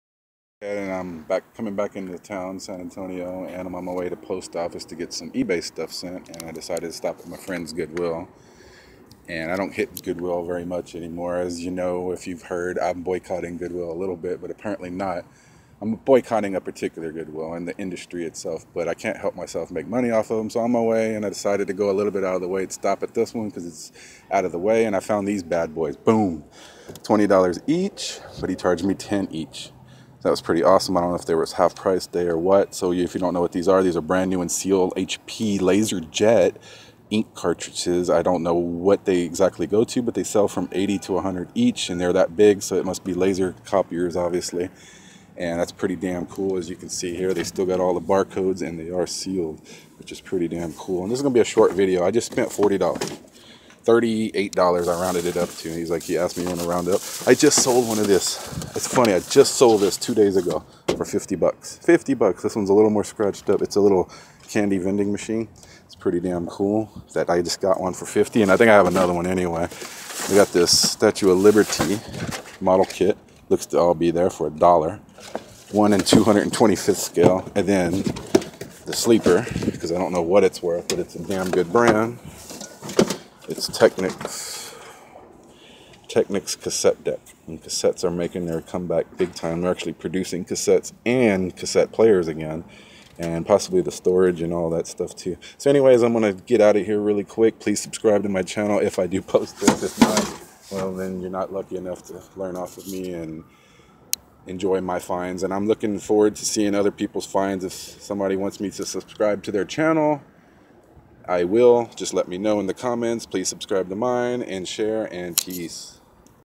Quick Last Minute Stop @ Goodwill & SCORE - ALPHA PICKER episode #38
Hey guys and girls whats happening? So I was on my way to the airport post office to get out some last minute ebay items. I was coming from a different direction which was going right past on of my favorite goodwill locations. I got there 10 minutes till closing time so I had to speed up my efforts. Spent $40 and scored pretty good, basically sold the 2 laser jet printing cartridges as soon as I posted them. did not realize how popular they were. Only paid $20 for the 2 new in box cartridges and sold them for $250! Couple of other cool items as well, overall id say it was a pretty good score in 10 minutes!